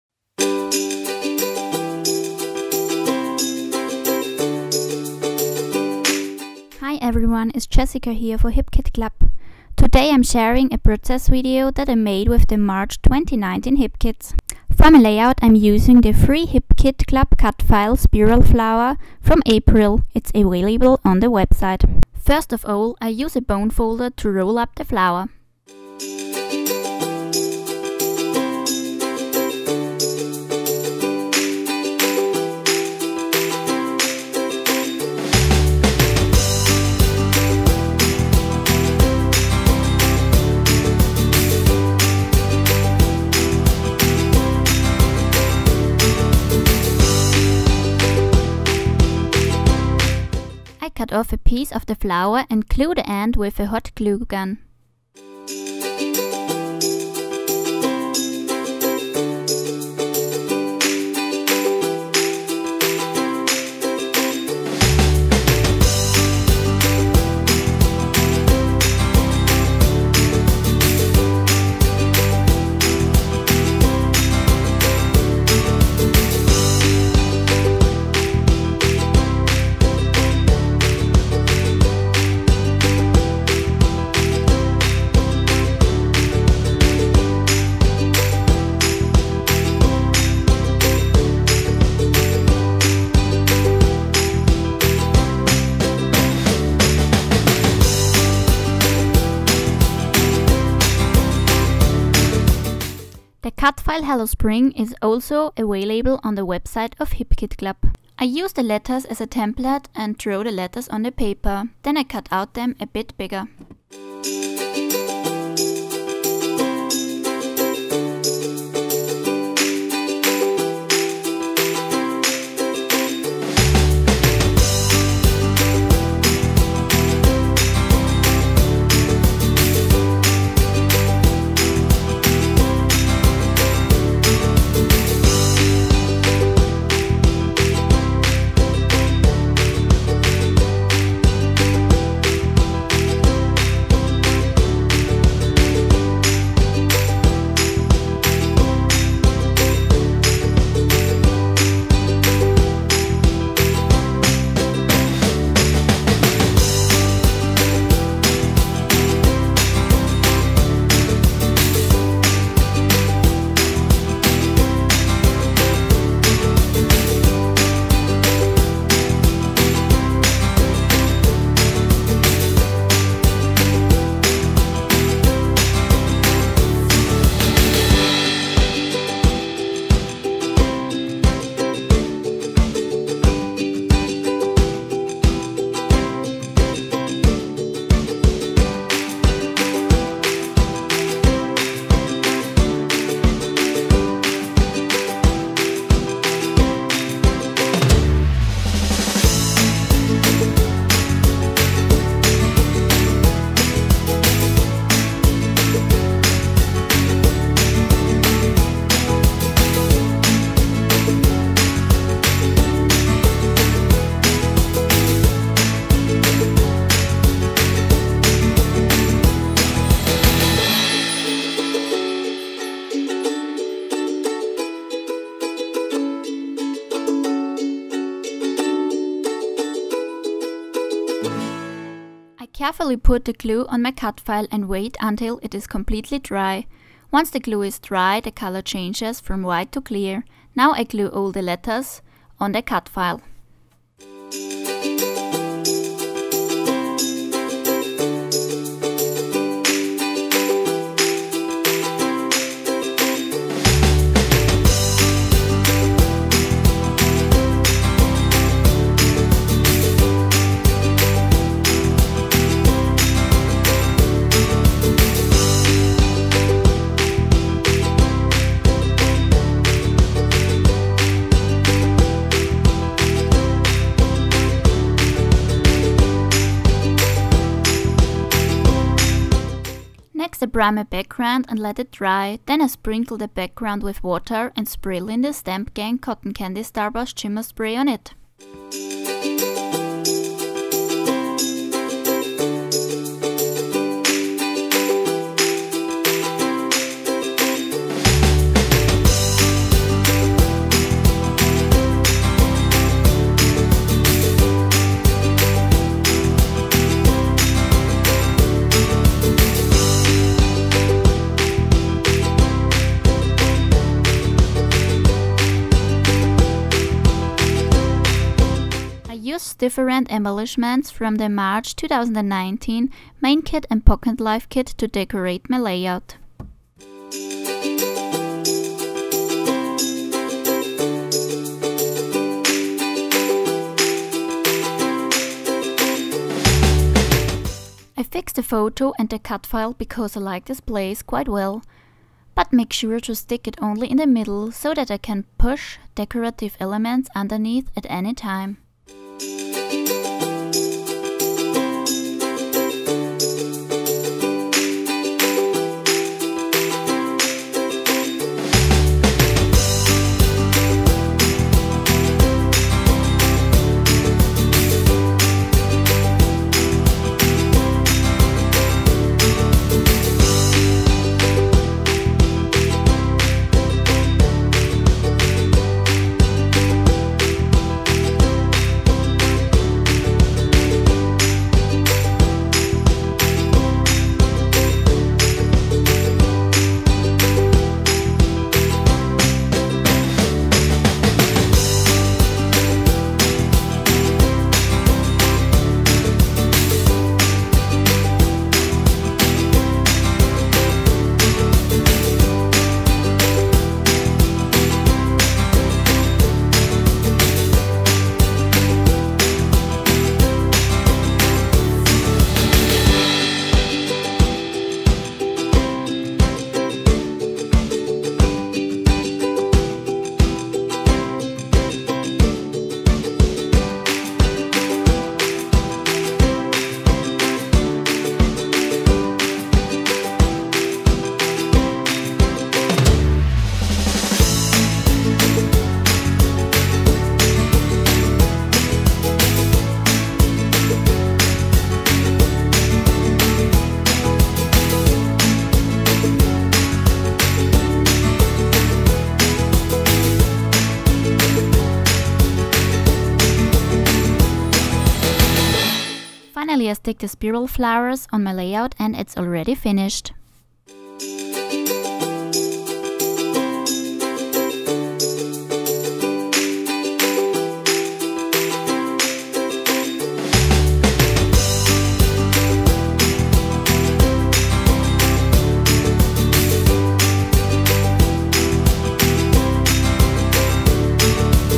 March 2019 HIP KIT CLUB Scrapbooking Process Video | Jessica Winter
Hi everyone! Jessica here today with a 12x12 scrapbook page using the Hip Kit Club March Main Kit and the March Pocket Life Kit.